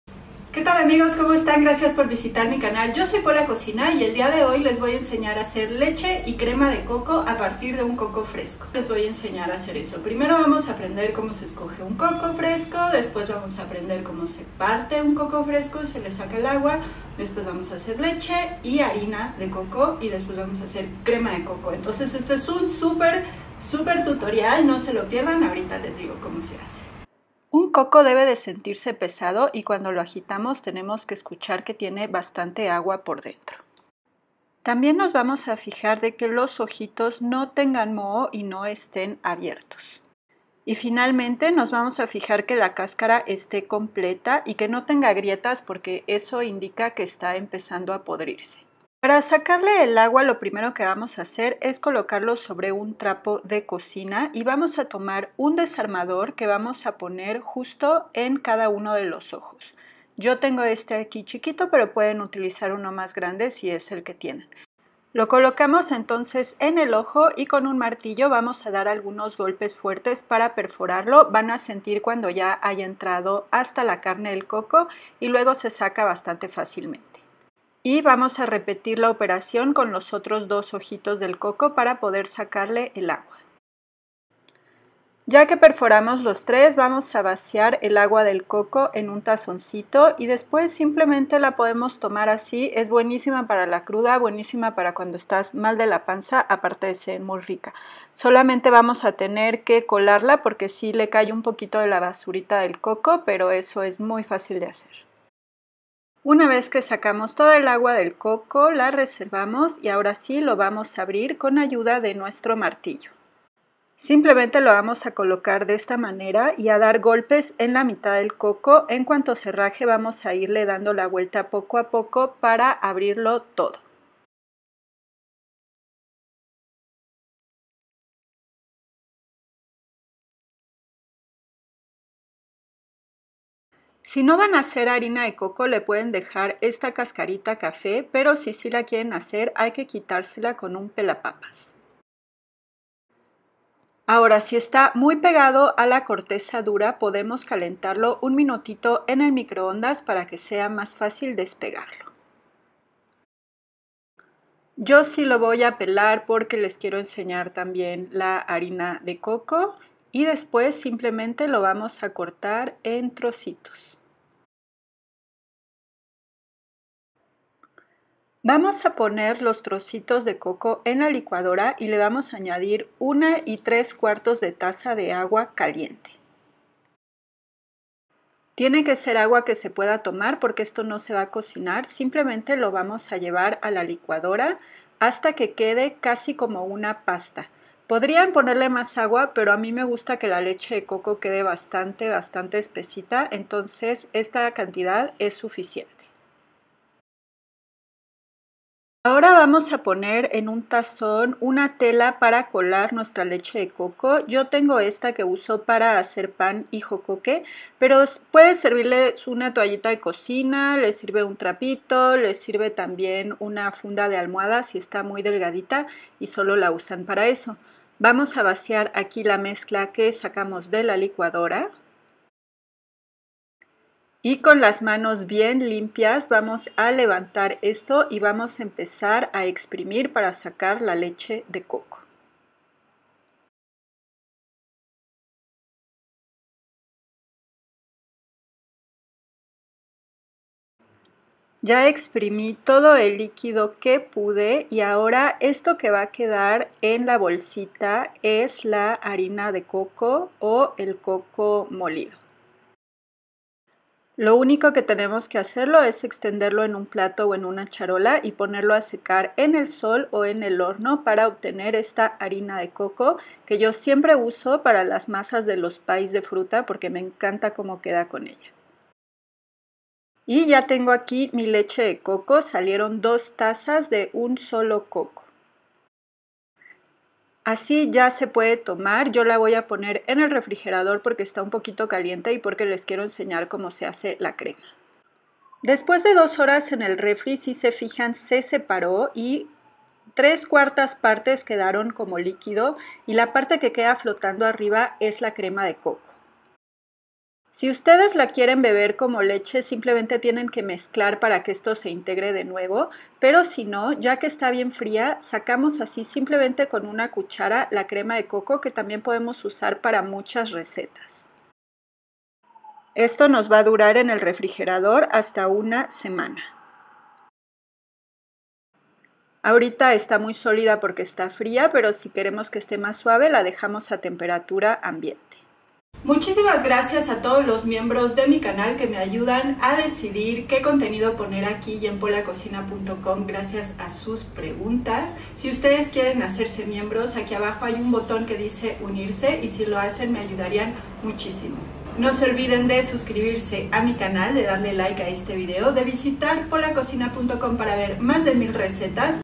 Cómo hacer leche de coco casera, Cómo pelar un coco, cómo hacer harina de coco, crema de coco fresco
Cómo hacer leche crema y harina de coco con un coco fresco. Cómo pelar un coco y sacarle el agua y la carne.
0:00 - intro
0:35 - cómo elegir un coco fresco bueno
0:57 - cómo sacar el agua de un coco
1:59 - cómo abrir un coco fresco
2:20 - cómo pelar un coco
2:56 - cómo hacer leche de coco casera y harina de coco
4:54 - cómo hacer crema de coco
5:33 - outro